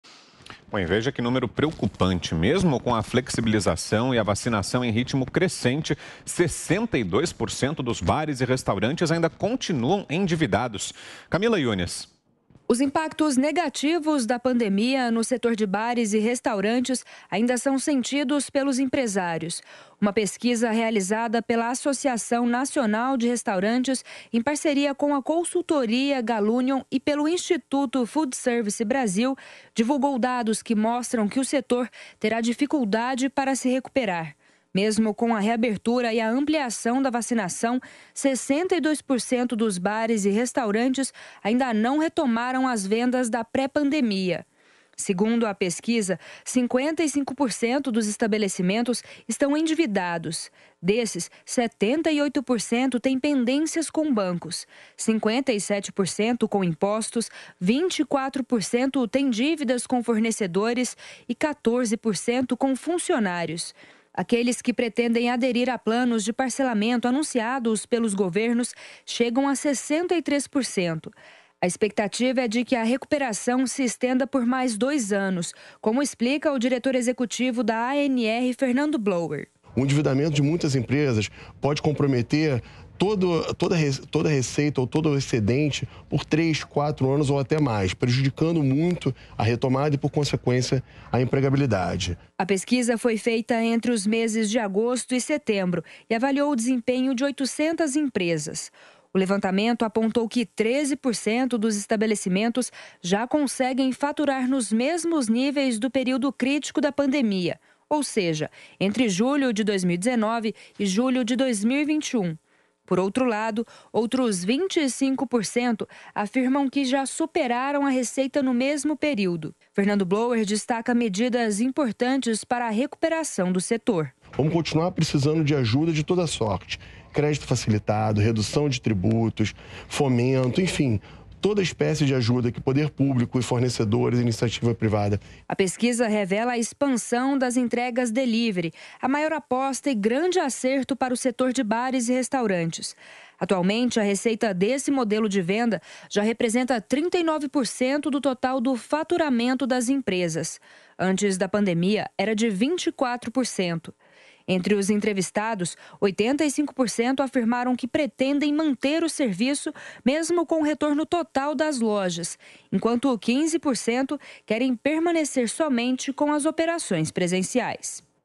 Apesar do avanço da vacinação, bares e restaurantes continuam no vermelho
Os impactos negativos da pandemia no setor de bares e restaurantes ainda são sentidos pelos empresários. Uma pesquisa realizada pela associação nacional de restaurantes divulgou dados que demonstram que o setor terá dificuldade para se recuperar, apesar das flexibilizações e avanço da vacinação.